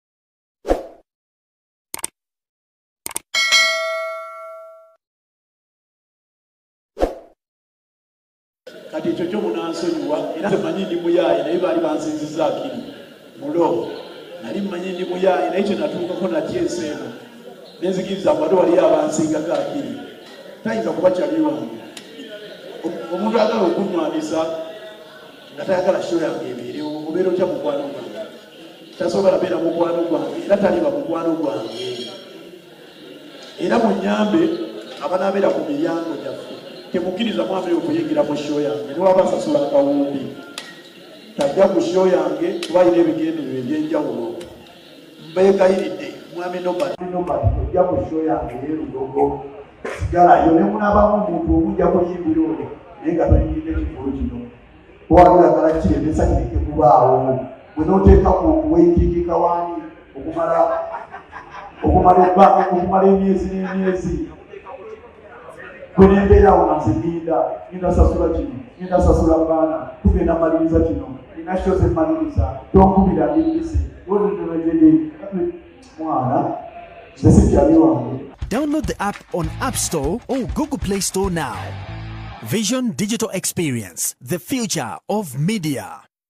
Wakiri ngy'akuyimbira omuntu omu naye nga Nobert talinye mu kivvulu kyange, kale nebwaba asasula
Wakiri ngy'akuyimbira omuntu omu naye nga Nobert talinye mu kivvulu kyange, kale nebwaba asasula kawumbi - GRAVITTY OMUTUJJU